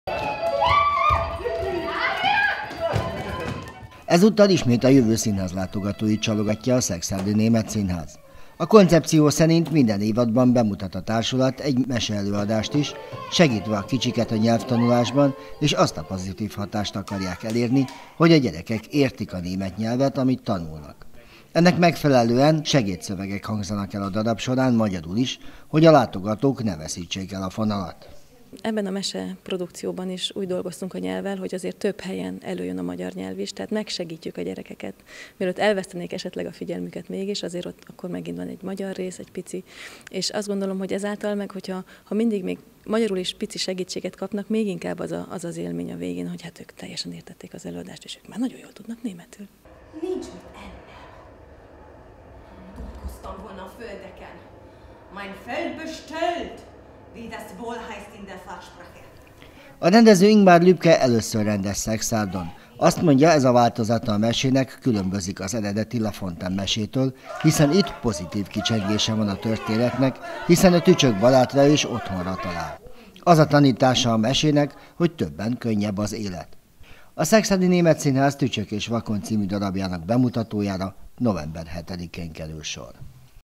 Bemutató-Gyerekdarabot vitt színre a Szekszárdi Német Színház v20181105
Új darab bemutatójára készül a Szekszárdi Német Színház. Lafonten Tücsök és a hangya című meséjének alapötletéből készült a Tücsök és a Vakond történetét színre vivő előadás. A bemutatóra november 7-én kerül sor.